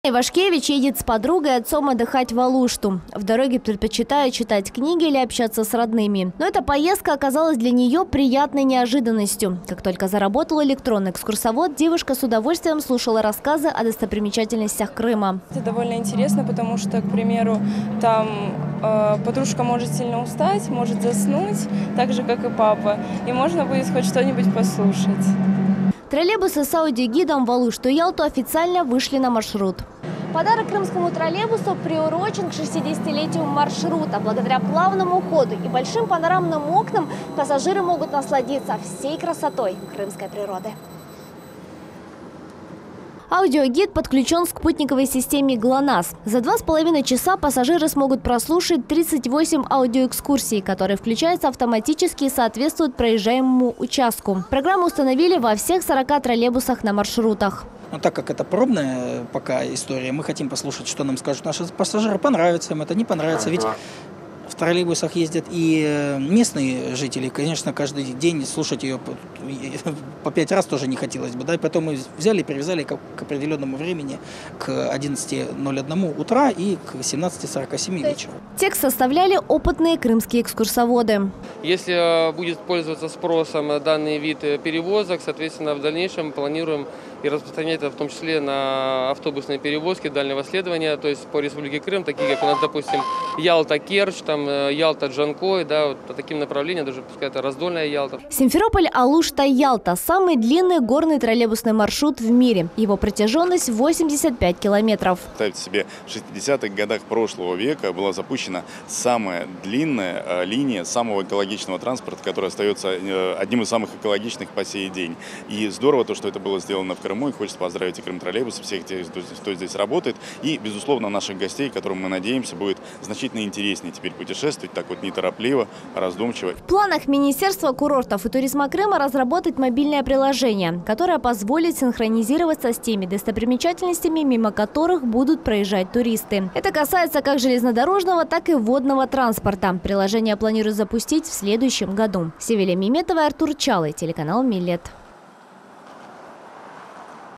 С аудиогидом – на ЮБК
С аудиогидом из крымской столицы на Южный берег. Ежедневно в Ялту и Алушту курсируют троллейбусы с необычным сопровождением. За два с половиной часа пассажиры могут прослушать 38 аудиоэкскурсий, которые включаются автоматически и рассказывают об участках, по которым проезжает пассажир. Подробности у Севили Меметовой.